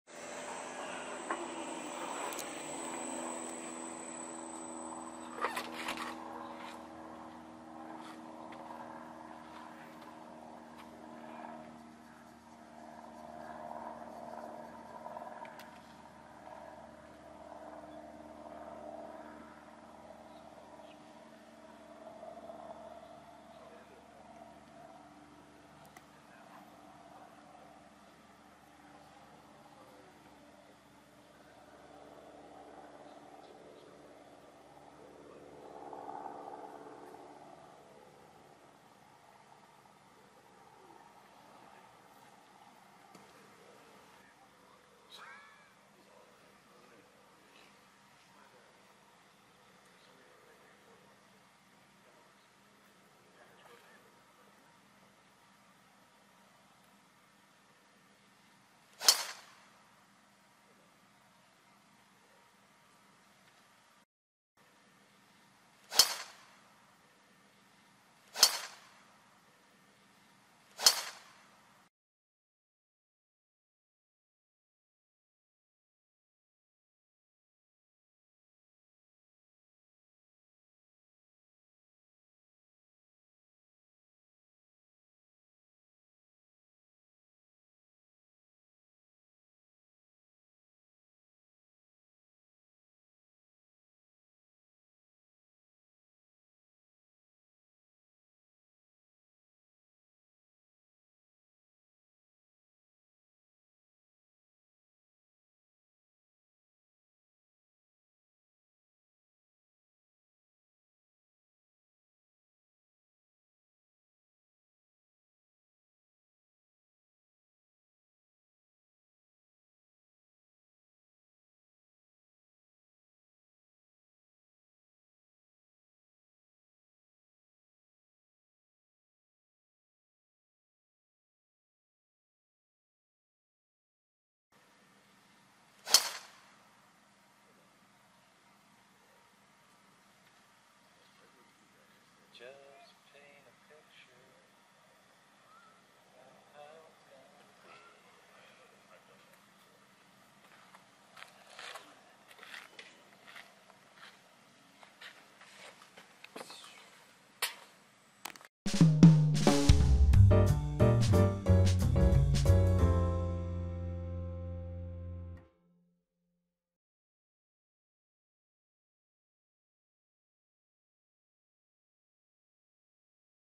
ジェームス・ハーン James Hahn【Driver Shot】2022 The American Express
アメリカ出身の男子プロゴルファー、 ジェームス・ハーン James Hahn  選手のドライバーショット映像。
後方からのスイング動画。
ルーティーンからフィニッシュまでの動画。
スローモーションとストップモーション動画。